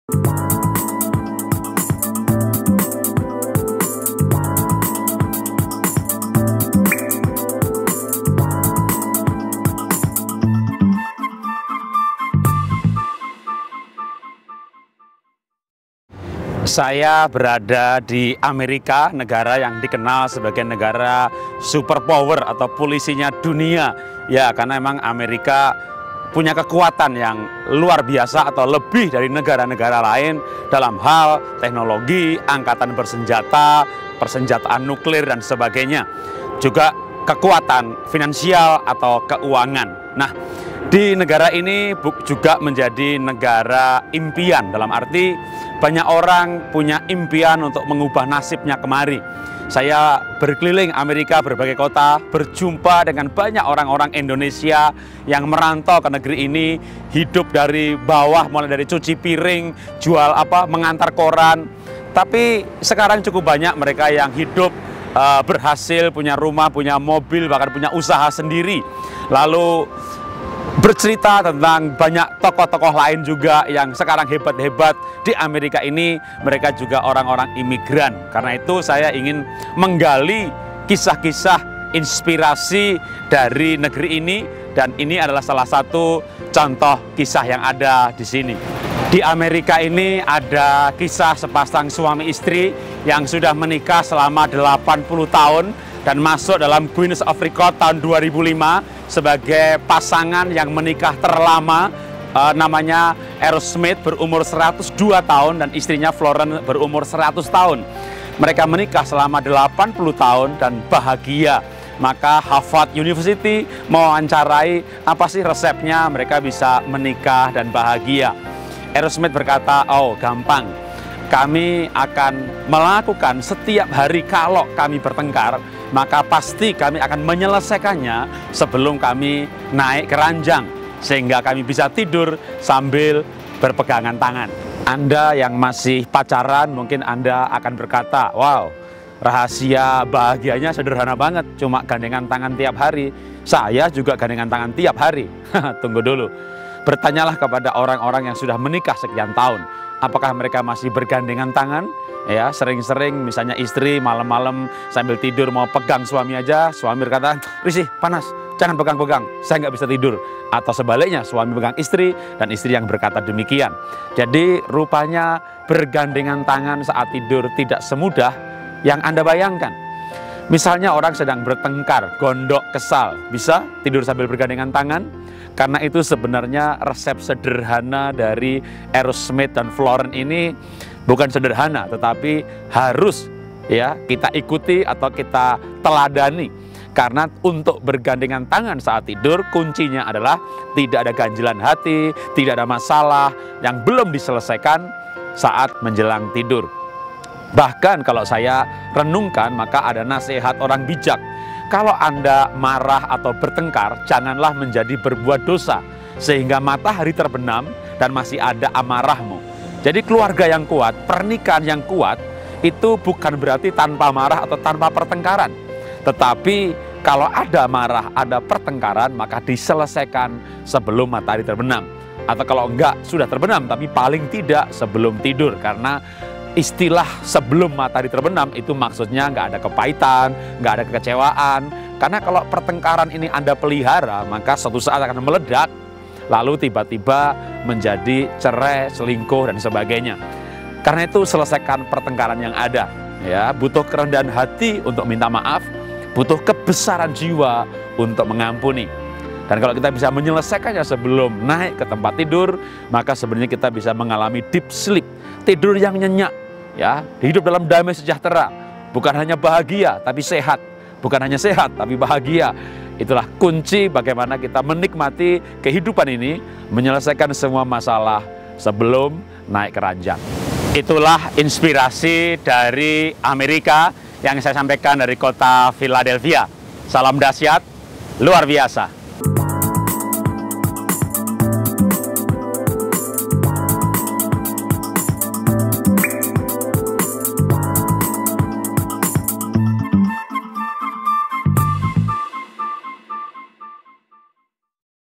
Inspirasi USA, Menikah Bahagia Resep Arrowsmith & Florence, oleh Ir. Jarot Wijanarko
Arrow S & Florence Pasangan yang sudah menikah 80 tahun dan bahagia, resepnya layak kita simak, ia menikah usia 20an dan sekarang usia 100an, dengan pasangan yang sama. ia berhak menasehati kita. caranya? Tidurlah dengan bergandengan tangan! Ha! Simple sekali! Tidak juga, apakah anda bisa tidur bergandengan tangan saat kecewa? saat bertengkar?

Karena itu, untuk menjalani nasehat ini, ikuti tipsnya yaitu "Jika harus bertengkar, bertengkarlah, tetapi jangan membawanya ke tempat tidur' "Sebelum matahari terbenam, maka amarah, perselisihan harus sudah diselesaikan" Tidak diungkit esok harinya. masalah hari ini diselesaikan hari ini. Hiduplah tanpa kekecewaan. Bersyukurlah dan cintai pasangan anda. Simak tips lengkapnya dalam Video HANYA 5 MENIT ini.

Semoga mjd berkah
Jarot Wijanarko